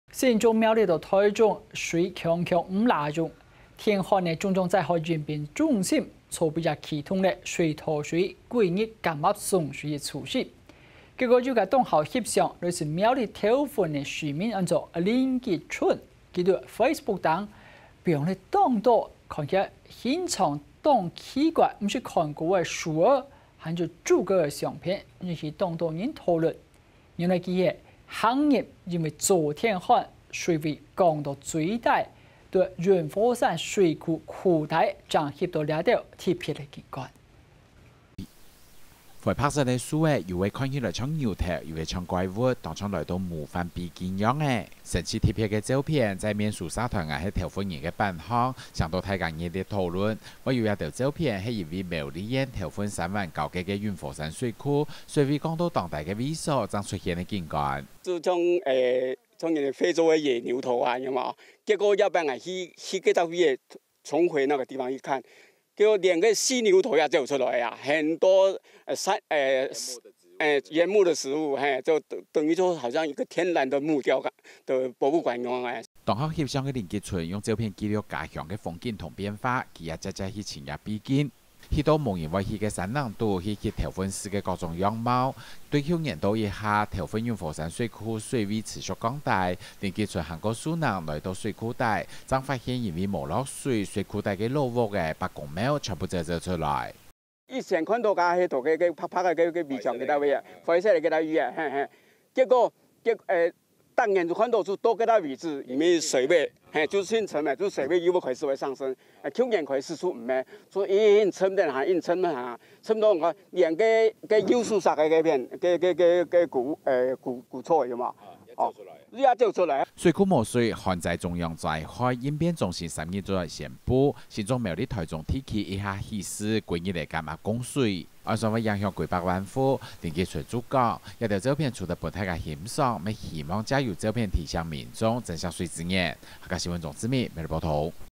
永和山水庫水位再探底 市民捕捉特殊景觀【客家新聞20210304】
竹苗中地區水情嚴峻，旱災中央災害應變中心啟動自來水全日減壓供水措施，熱愛攝影的苗栗縣頭份市民林桔春，最近在臉書上po出一系列，形狀奇怪的枯木、枯竹照片，紛紛引起民眾好奇、討論，原來這是他走進，因為乾旱、水位驟降的永和山水庫的庫底，捕捉到的特殊景觀。